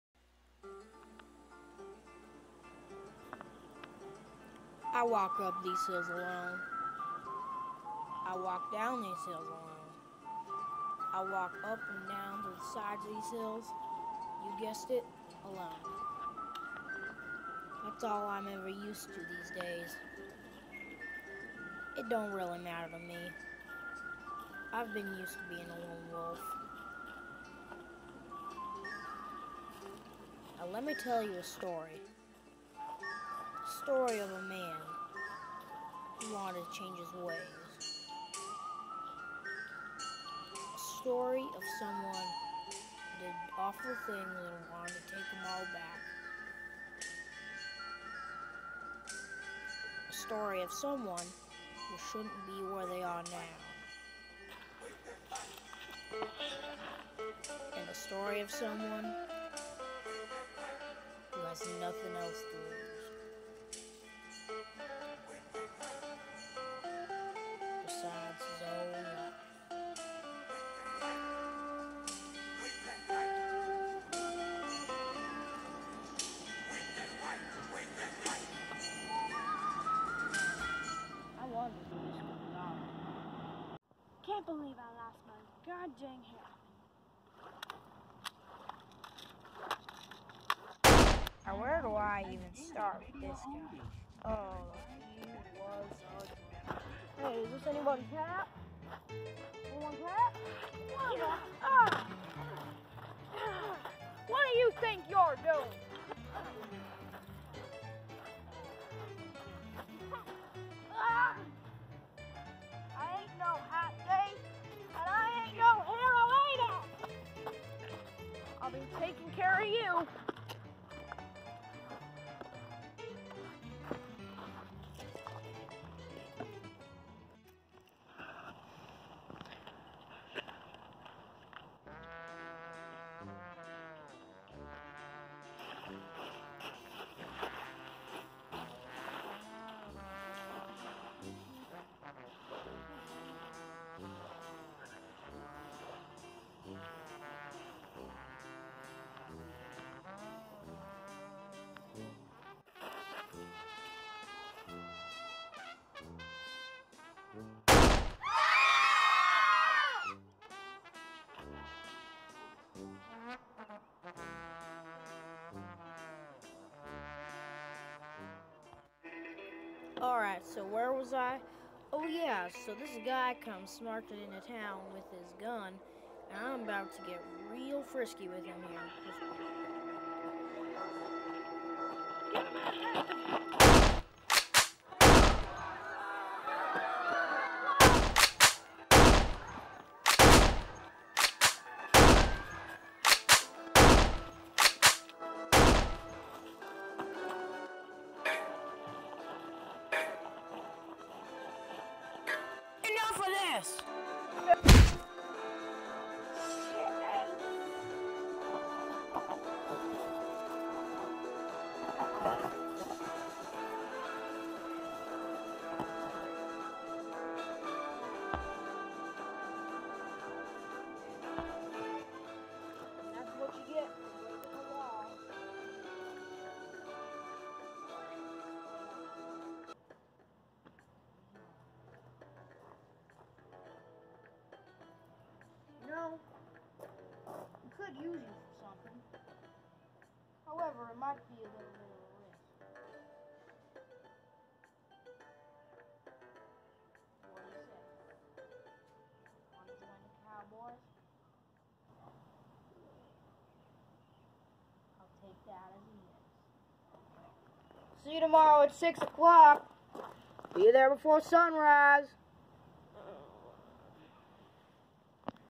Black Hawk and White Ghost episode 1: The Drunk and the Ranger
This is a small, Western themed mini series called Black Hawk and White Ghost! There will be 5 episodes.

Philip Rodgers is the town sheriff, with a heart of gold. Steven (Charlie) Harvey is the town drunk, nobody ever nods a eye at his selflessness. The two will come toghtether to become a crime-fighting, force to be reckoned with. But with every story, comes a meeting moment...